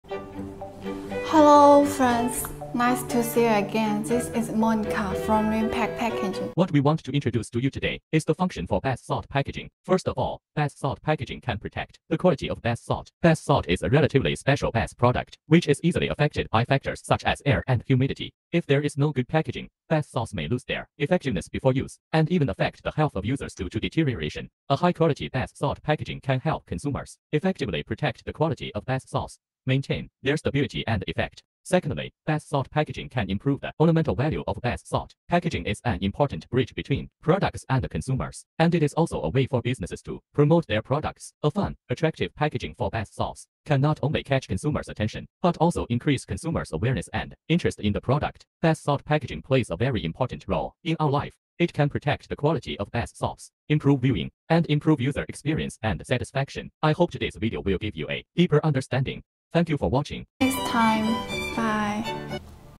How to Custom Bath Salt Packaging | Recent Making Record | Kindly Visit
Hello everyone, what we want to introduce to you today is the function for bath salt packaging

First of all, bath salt packaging can protect the quality of bath salt. Bath salt is a relatively special bath product, which is easily affected by factors such as air and humidity. If there is no good packaging, bath salts may lose their effectiveness before use, and even affect the health of users due to deterioration. A high-quality bath salt packaging can help consumers effectively protect the quality of bath salts, maintain their stability and effect.

Secondly, bath salt packaging can improve the ornamental value of bath salt. Packaging is an important bridge between products and consumers, and it is also a way for businesses to promote their products. A fun, attractive packaging for bath salts can not only catch consumers' attention, but also increase consumers' awareness and interest in the product.

Bath salt packaging plays a very important role in our life. It can protect the quality of bath salts, improve viewing, and improve user experience and satisfaction.

-Low MOQ & Fast delivery
-Innovative Packaging Solutions
-Maximize your brand impression
-Unbeatable Lead Time
-We're all about our clients